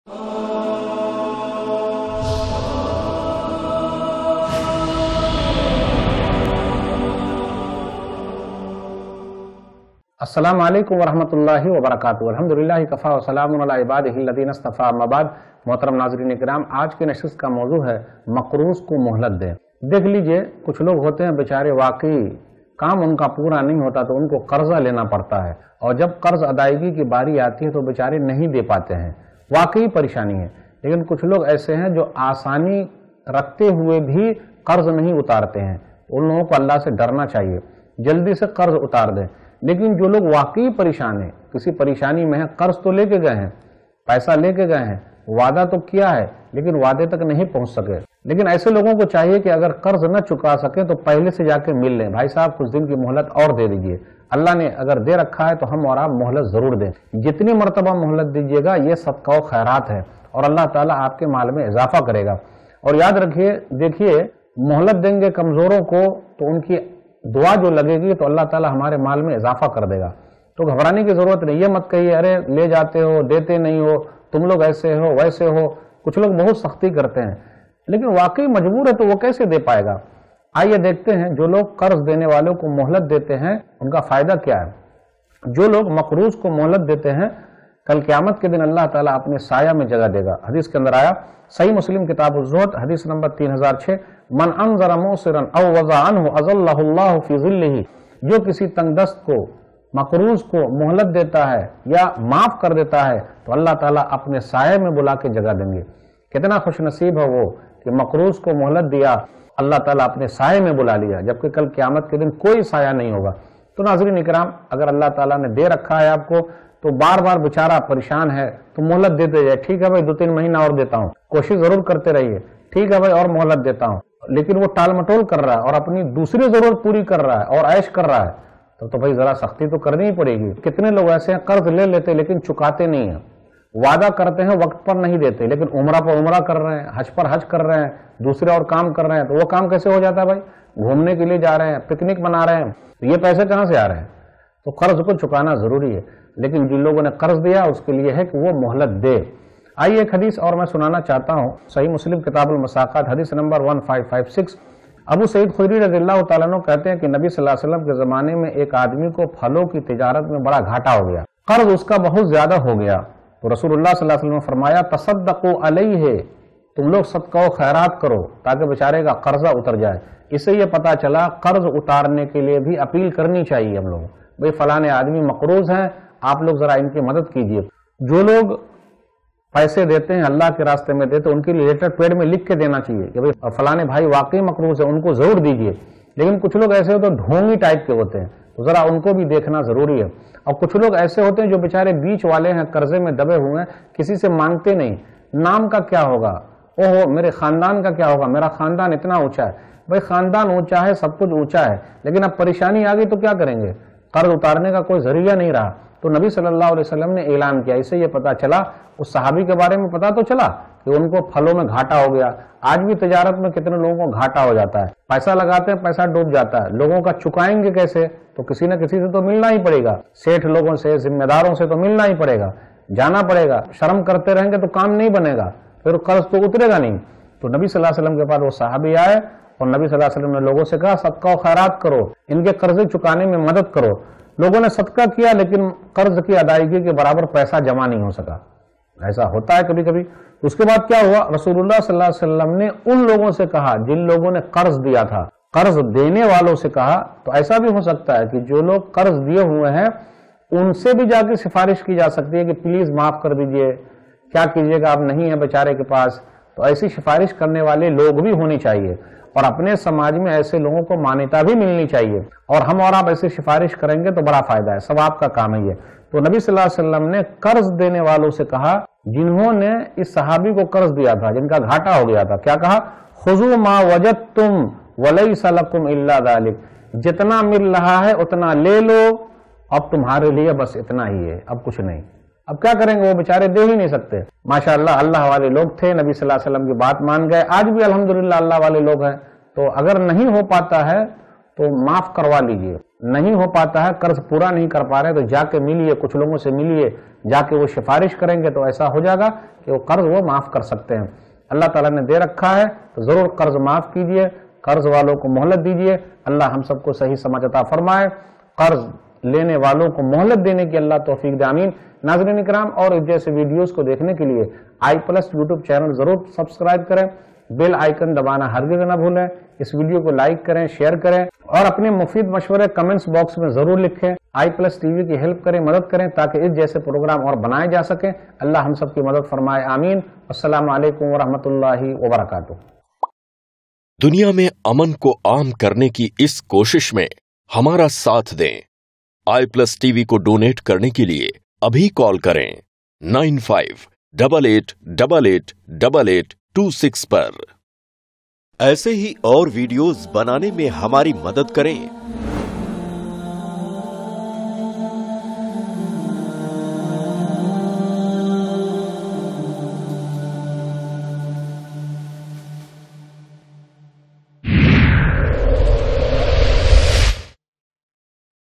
Qarzdar Ko Mohlat Dein, Kyunki..... | Ek Nasihat 93 | Shaikh Sanaullah Madani | iPlus TV
QARZ ADAYEGI ME MOHLAT YA FIR QARZ MAAF KAR DENE PAR KYA HOTA HAI AAIYE DEKHTE HAIN IS VIDEO MEIN, KYA KARZ KI ADAYEGI KE LIYE APPEAL KAR SAKTE HAIN AAIYE JANTE HAIN, EK BAAR EK BUSINESS SAHABI THE UNKO BUSINESS ME GHATA HO GAYA UNPAR QARZ KA BOJH BADH GAYA TO ALLAH KE RASOOL NE UNKI KAISE MADAD KI AAIYE DEKHTE IS INTERESTIN VIDEO MEIN

UPI : donateilm@icici